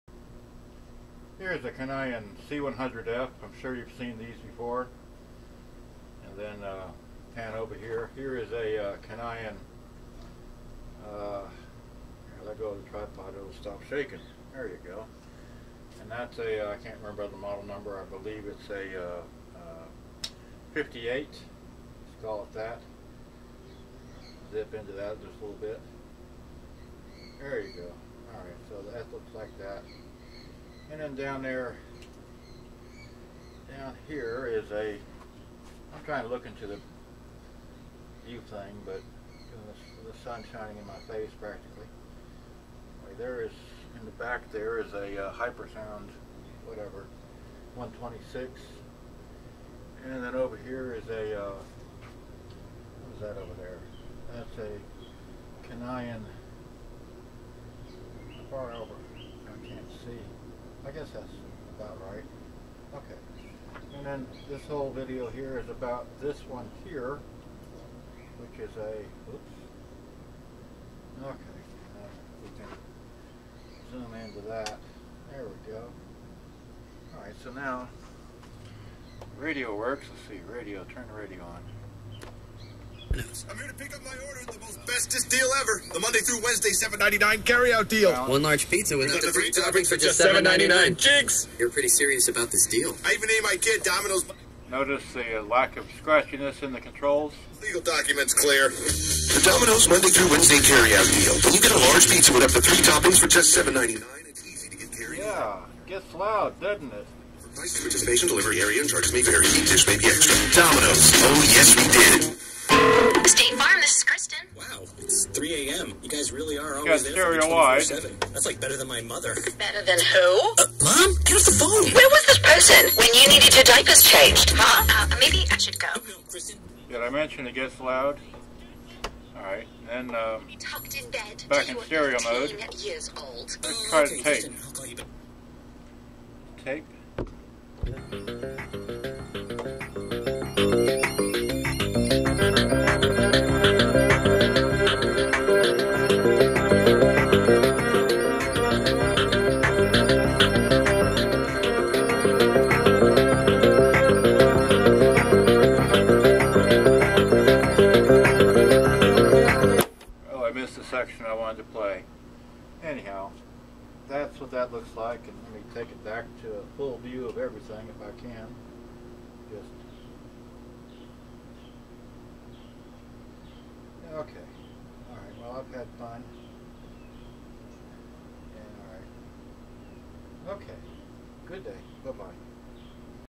Conion CRC-H84FC Boombox gets's Loud stereo wide radio then tape Panned Zoomed
Forgot to show how the drawer works - 00034 Conion C-100F, CRC-H58F, ( C-126F Super Jumbo 60 - Hyper Sound ), Conion Technisonic TC-999,
Conion CRC-H84FC gets's Loud stereo wide radio then tape